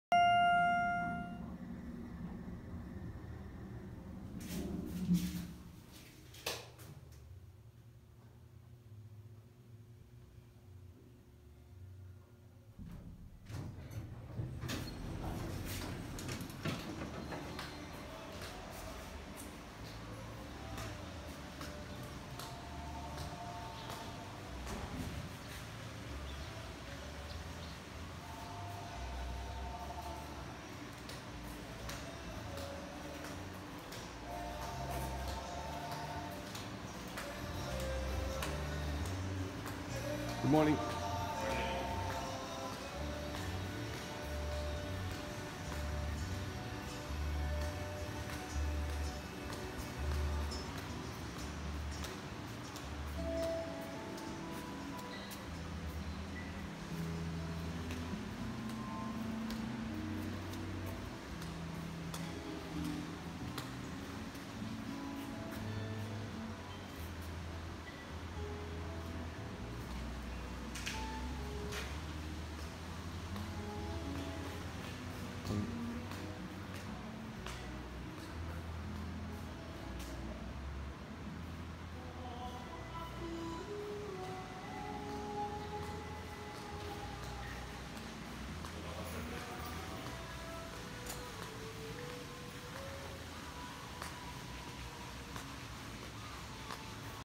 Maui Coast Hotel Kihei, Maui, Hawaii.
Lobby walk through of Maui Coast Hotel August 2018. Just a quick overview, not intended to be a professional review. Hopefully it will help future visitors,